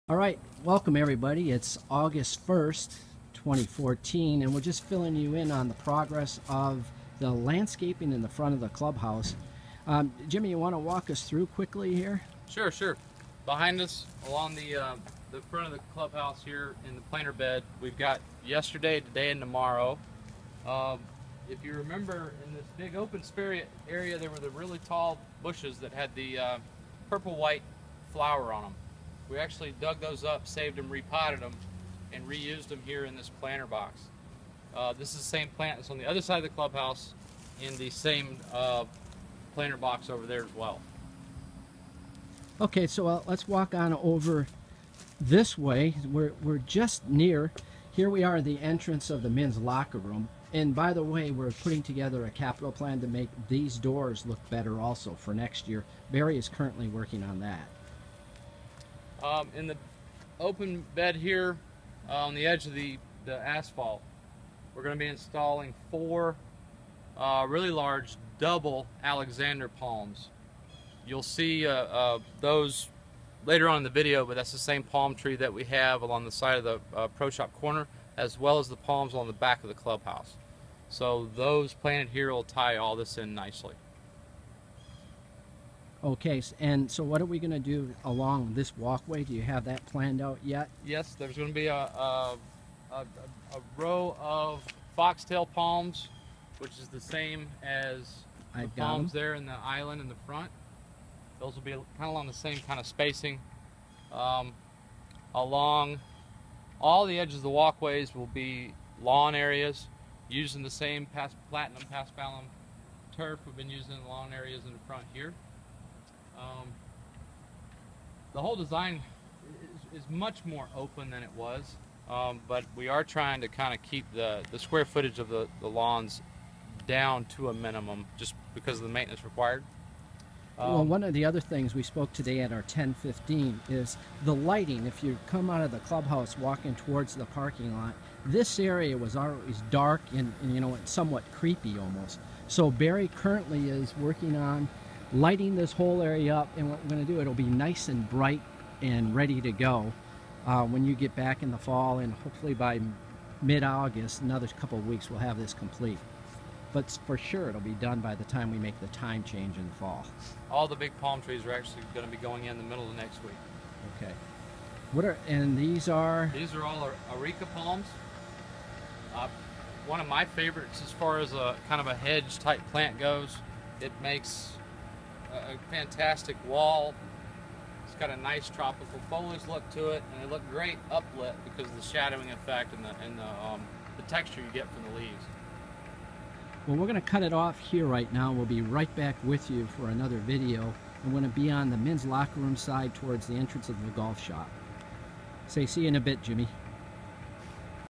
Clubhouse Landscaping Update (1/2) 8/1/14
Updated details on the landscaping design taking place in front of Eagle Creek's Clubhouse